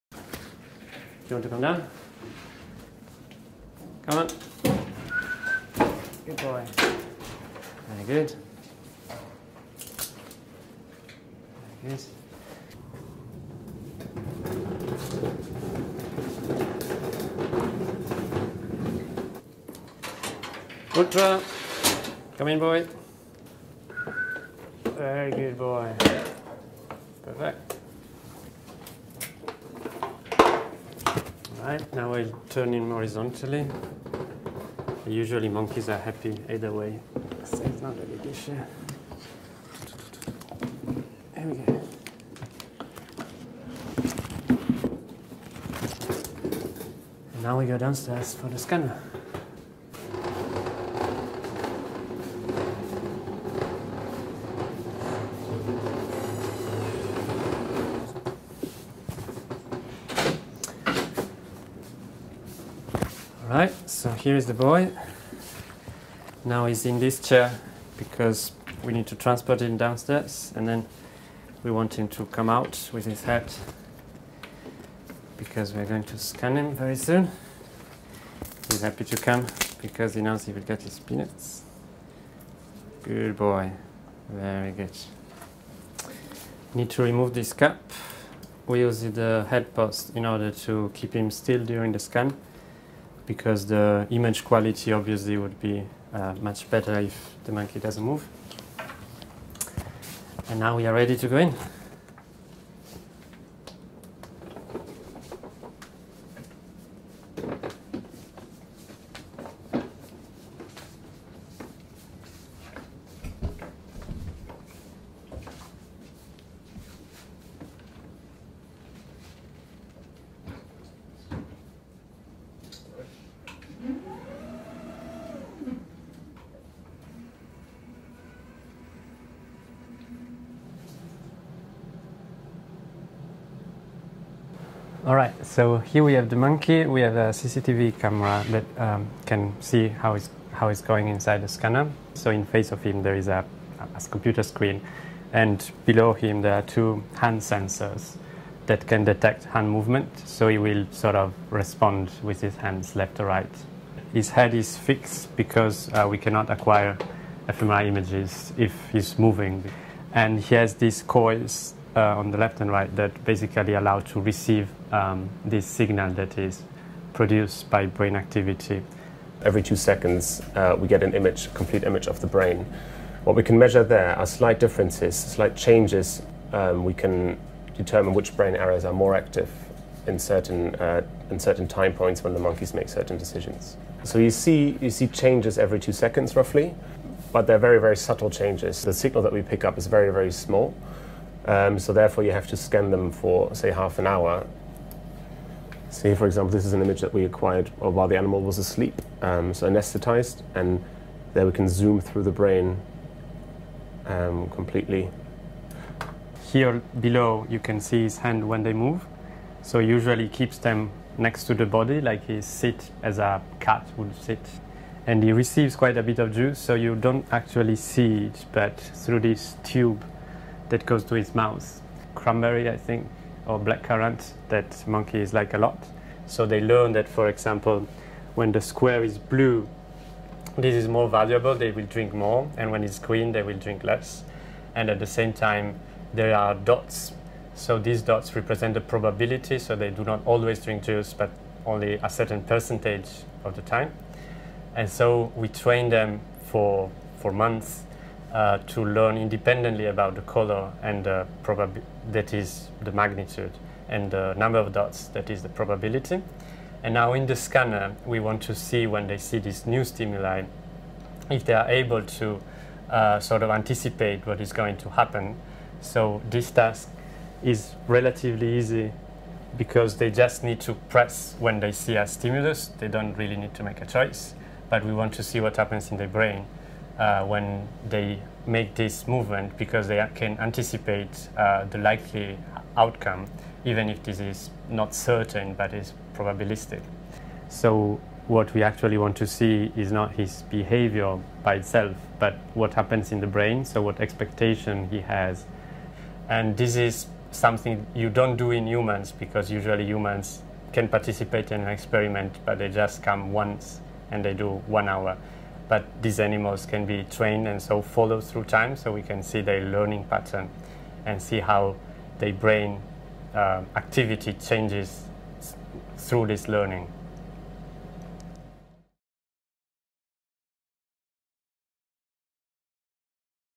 Measuring 'anticipation' in the brain
Macaques have a brain-scan while watching a computer that presents them with dots. This allows us to see where 'expectation' occurs in their brain.